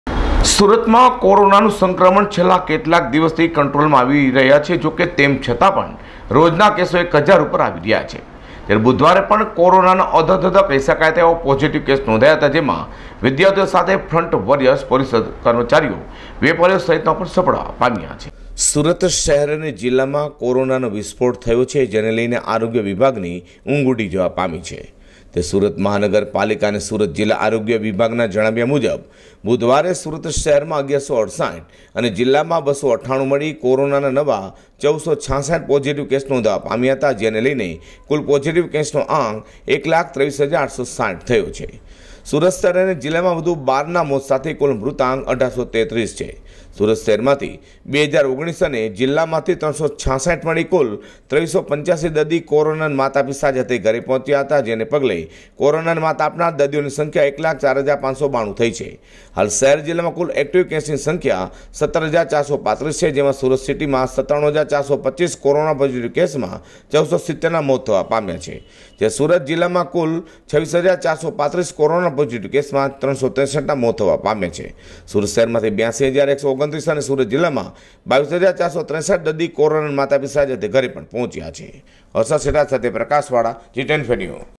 હાશ કોરોનાને હરાવી ૨૩૮૫ લોકો ઘરે પહોંચ્યા | G 24 NEWS
સુરતમાં કોરોનાના કુલ ૧,૨૩,૮૬૦ તો ડિસ્ચાર્ઝ ૧,૦૪,૫૯૨ જ્યારે મૃતાંક ૧,૮૩૩